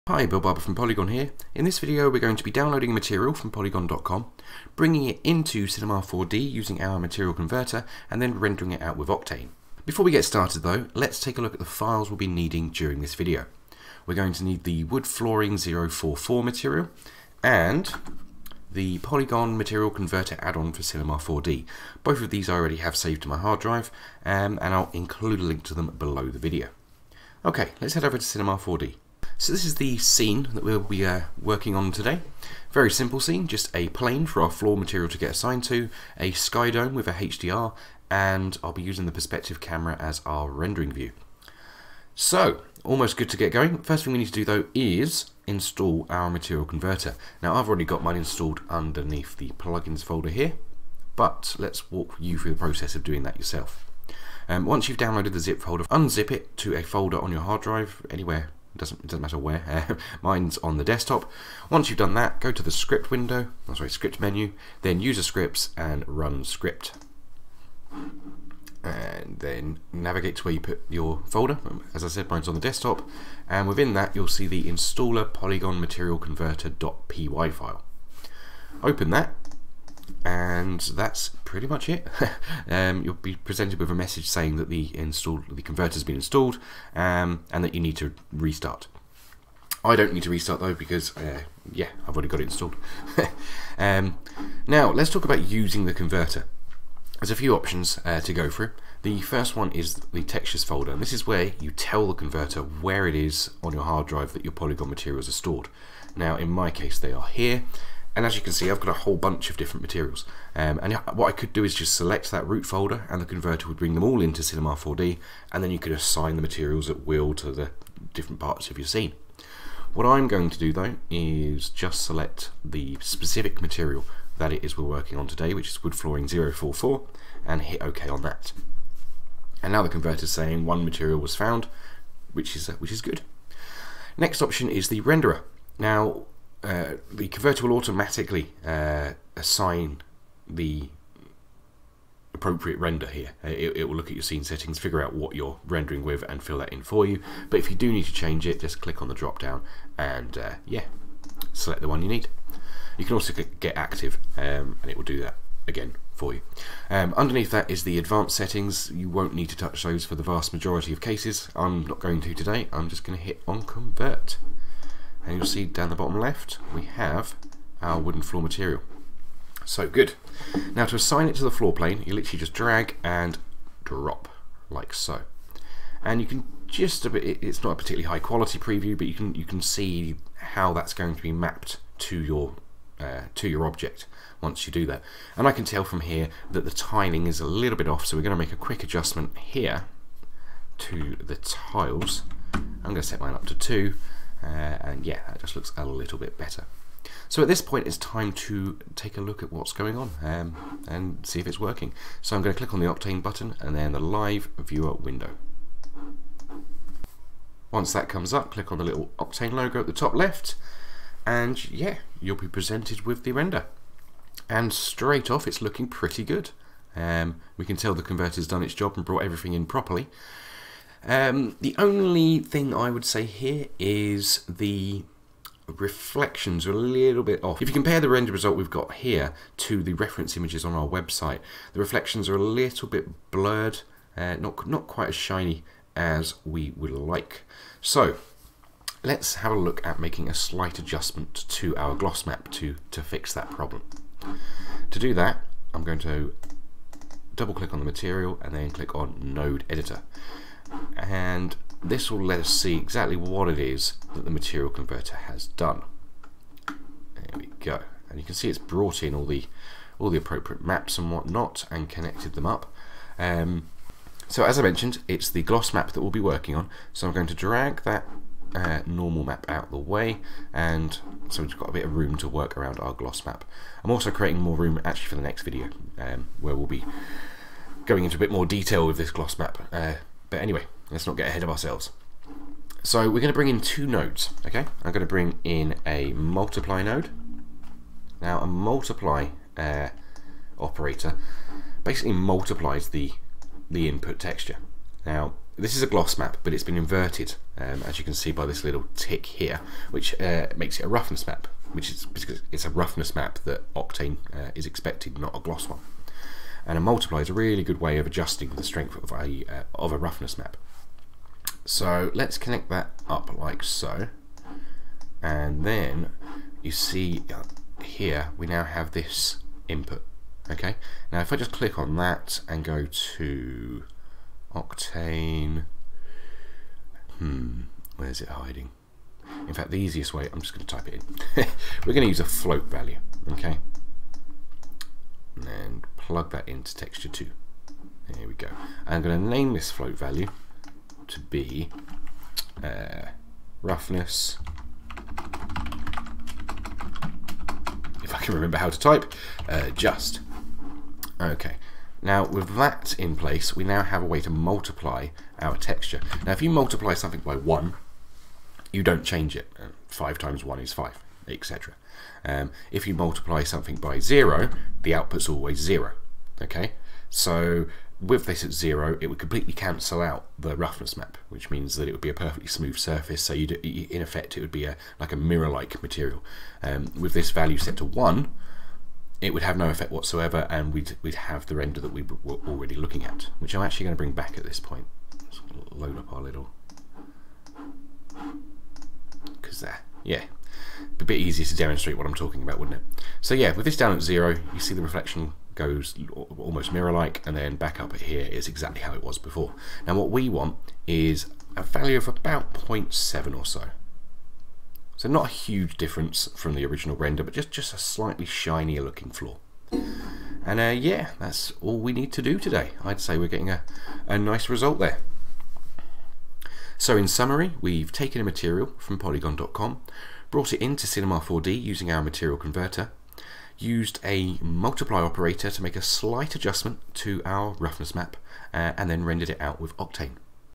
Using Poliigon textures in Cinema 4D with Octane
In this video we'll be taking a material from Poliigon.com, bringing it into Cinema 4D using our material converter and then rendering it out with Octane.